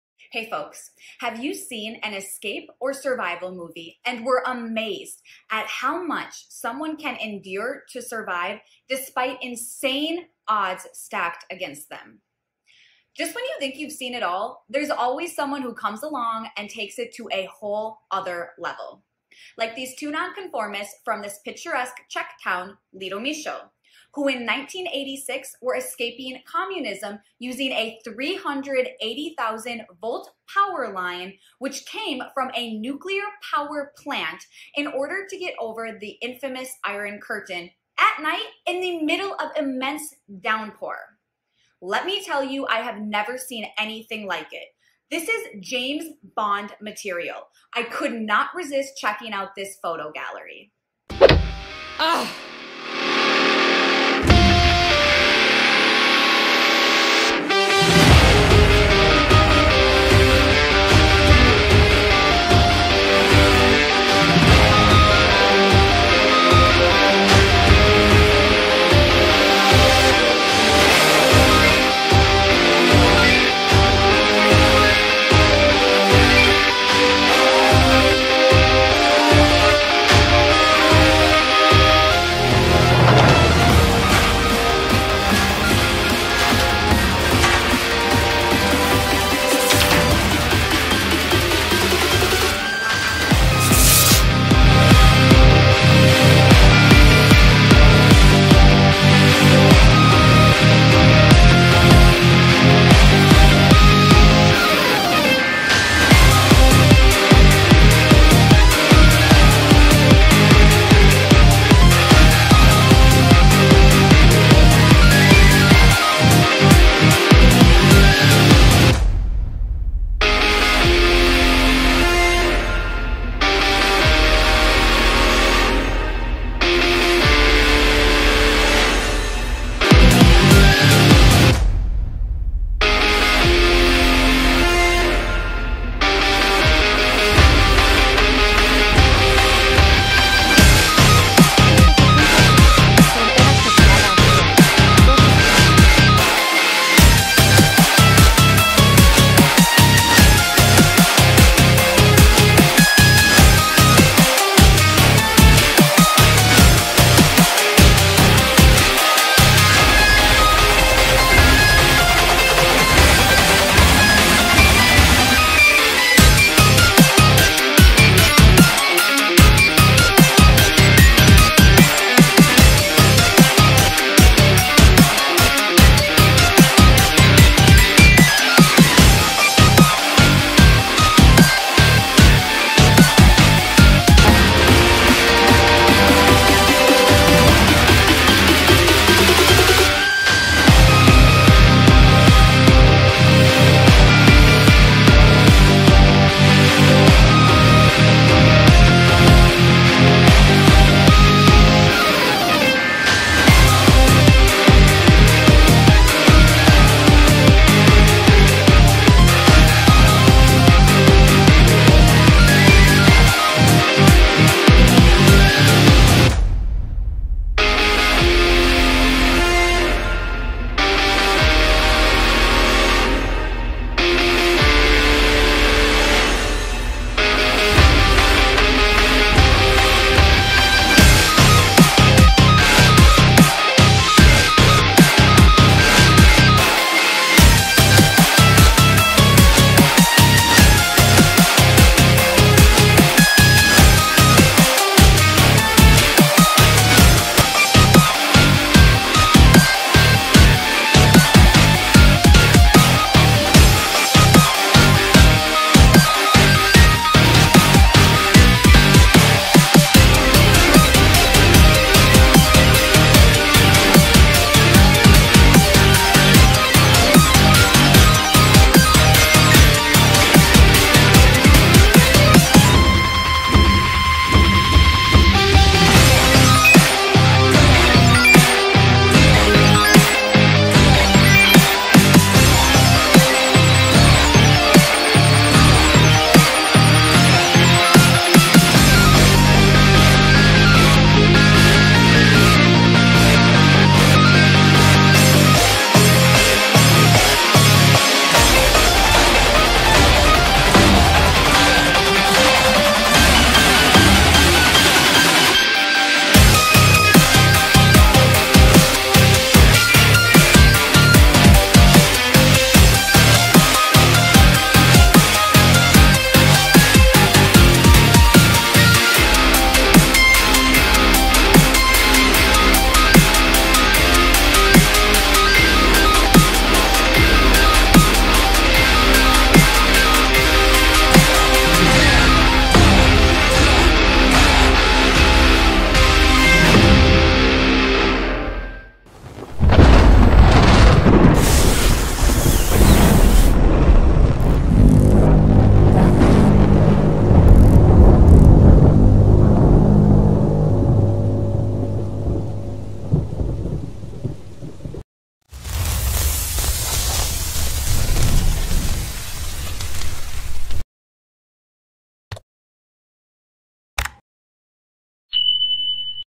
I SHOULDN'T BE ALIVE: High Voltage Power Line Used As An Escape Route. Cold War Story + 2 new pics.
1986 - Czech/Austrian border. Two nonconformists, in a small town called Litomysl, one of them a lumbar jack, a bit of a 'mad scientist' trying to discover free energy, the the blond one a railway mechanic. These two determined to humiliate the totalitarian regime, and broke out of the biggest Supermax in the world, which was Communism. Land mines, dogs, snipers and rows of coiled razor sharp barbed wire didn't stop them in their pursuit of freedom. They penetrated highly restricted, and militarized border area, and escaped by zip lining on a 380 kV power line that comes out of a nuclear power plant called Dukovany, over the deadly Iron Curtain, from former Czechoslovakia, into Austria,, and all that in a severe rainstorm. According to Western media, this was by far, the most electrifying, daring and certainly the most creative escape of Cold War era, with the level of intensity easily comparable to, if not surpassing gruesome escape stories from North Korea.
Truly, a unique triumph of a human spirit that would perfectly fit at least into "I Shouldn't Be Alive" TV documentaries.
For a complete photo gallery and pics of the zipline pulley that's displayed in Museum of Escapes, Checkpoint Charlie, in Berlin/Germany, go to the bottom of the second website below.
Likes are appreciated and comments welcomed.